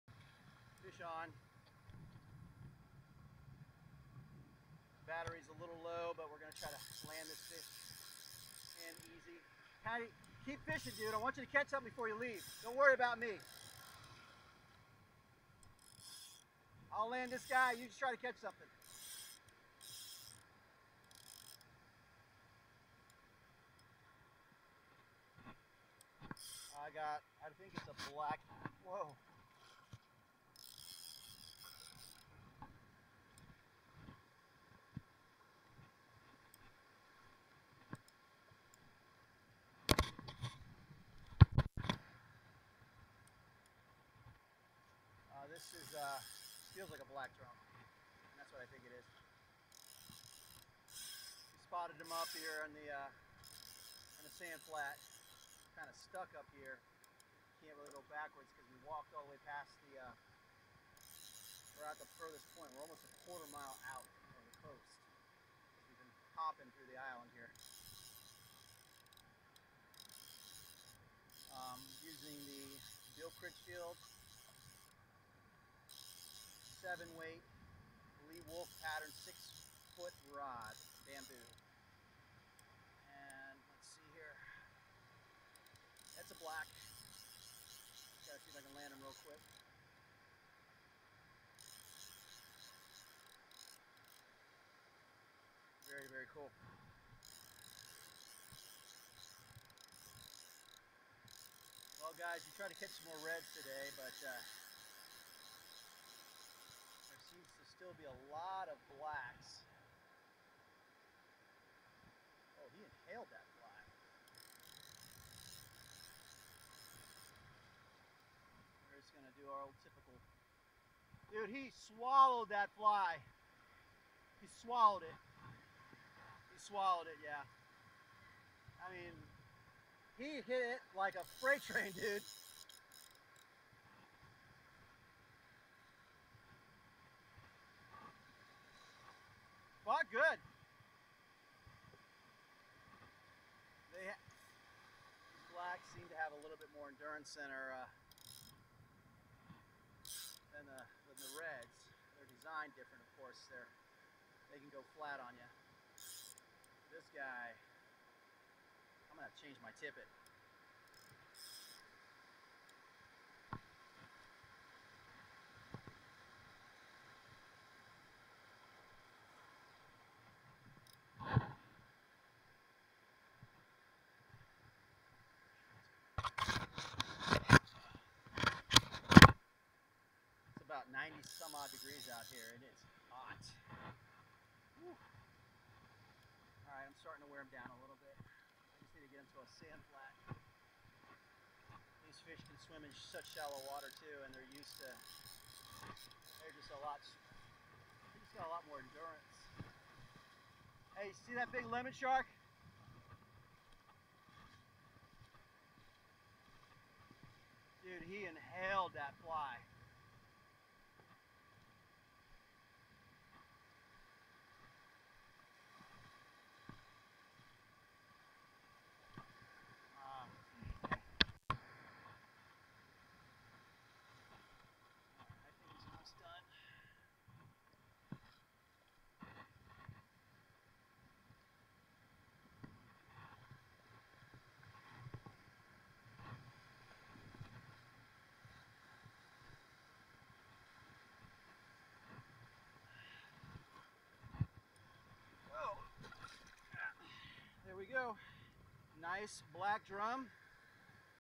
Saltwater Black Drum on a Lee Wulff Bamboo fly rod Taper by Bill Critchfield.
I caught this saltwater Black Drum on a 6ft 7wt Bamboo Fly Rod. Sorry, I was only able to get the last few minutes of this 8-minute catch. This is rod was originally designed for large trout and salmon. It was designed for bigger fish in fresh water. Today I tested it on Red and Black drums on the saltwater sand flats. It worked like a champ. With this bamboo rod design, I can cast all day and not get tired. The heat today is another story. Over 90 degrees and a little wind pushing through.